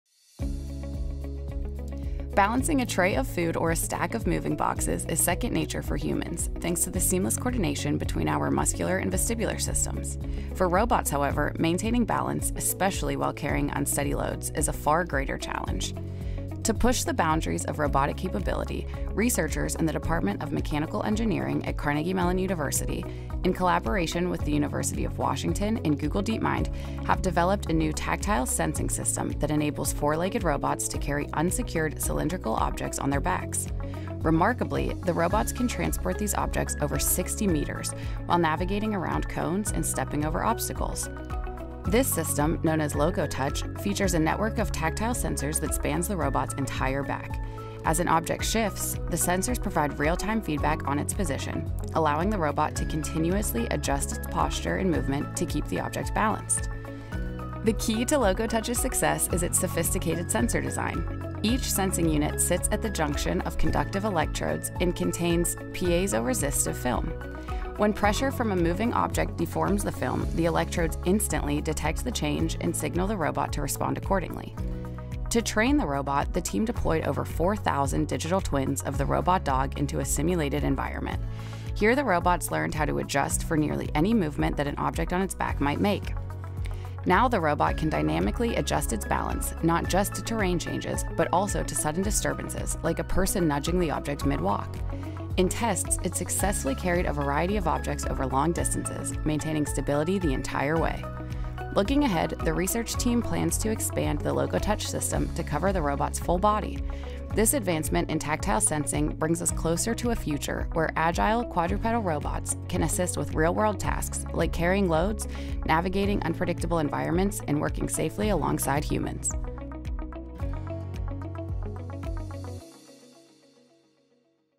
LocoTouch: Tactile Sensing Systems for Four-Legged Robots
To push the boundaries of robotic capability, researchers in the Department of Mechanical Engineering at Carnegie Mellon University in collaboration with The University of Washington and Google Deepmind, have developed a new tactile sensing system that enables four-legged robots to carry unsecured, cylindrical objects on their backs. This system, known as LocoTouch, features a network of tactile sensors that spans the robot’s entire back. As an object shifts, the sensors provide real-time feedback on its position, allowing the robot to continuously adjust its posture and movement to keep the object balanced.